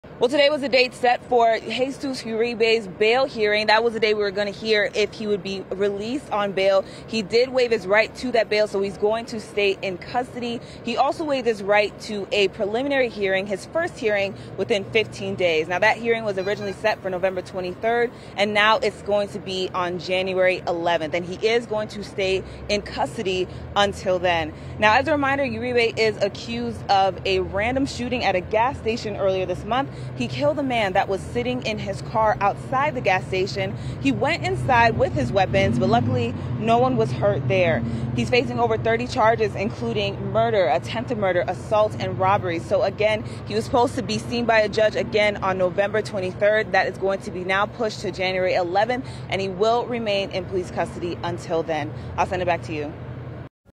'Random' shooting suspect waives right to bail hearing in Las Vegas court
Jesus Uribe, the 22-year-old man suspected in a deadly gas station robbery, waived his right to a bail hearing on Monday.